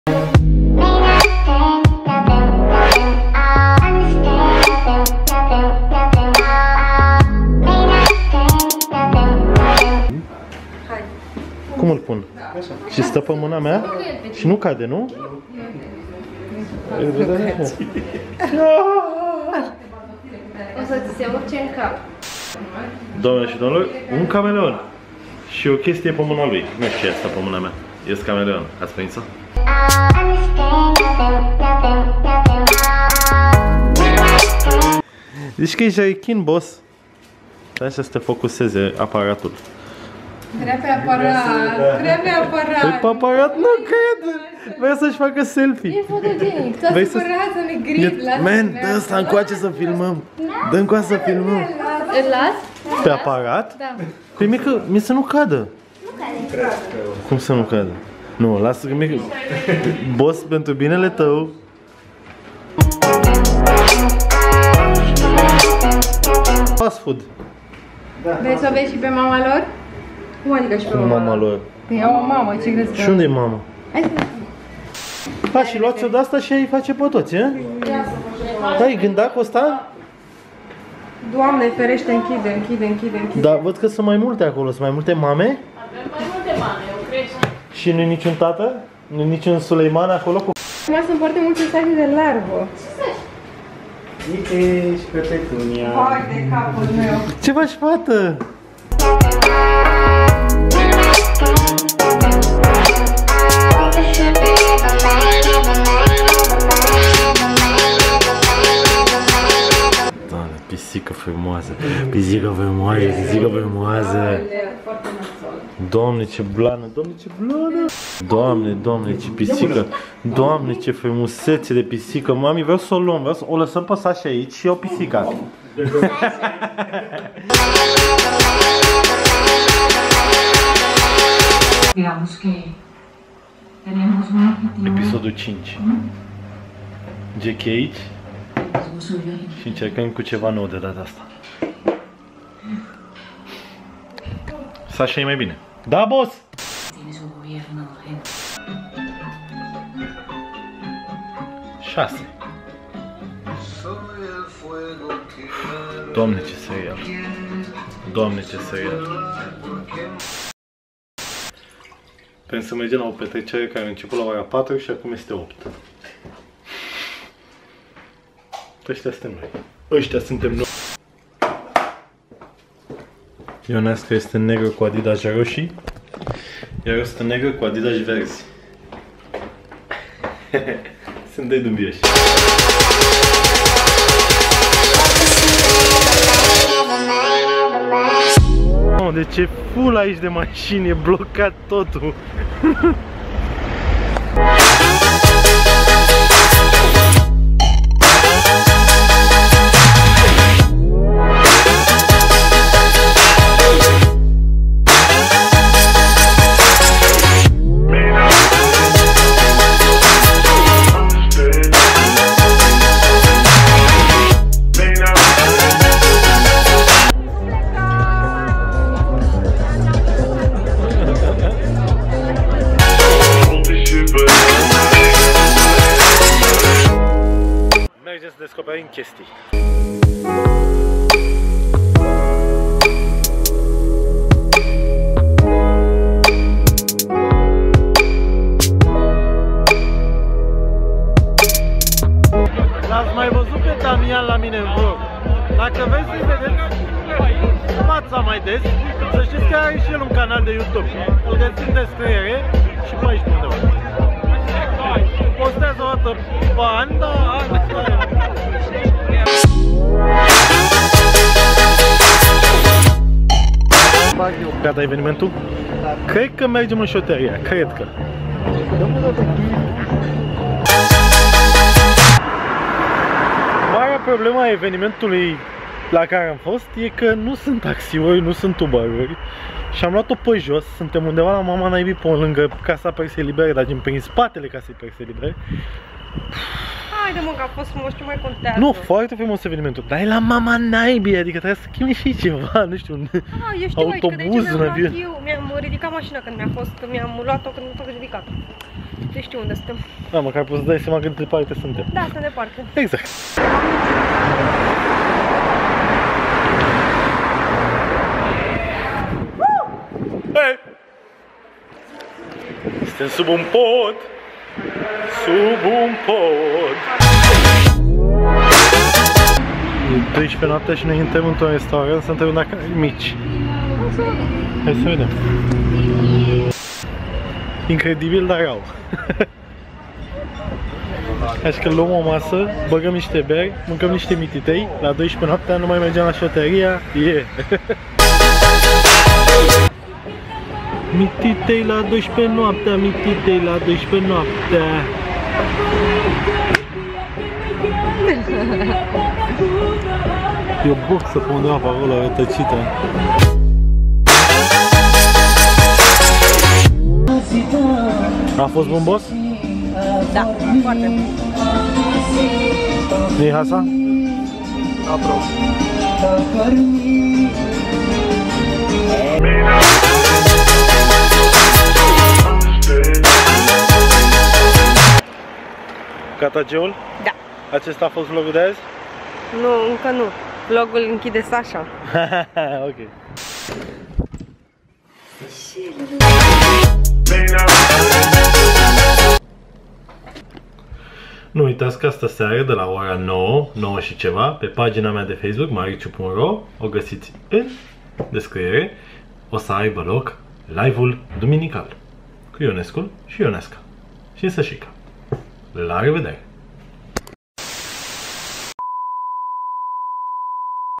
S-a urcat un cameleon pe mine
În episodul zilei mă împrietenesc cu tot felul de reptile, iar seara merg asortat cu Ioneasca la o petrecere.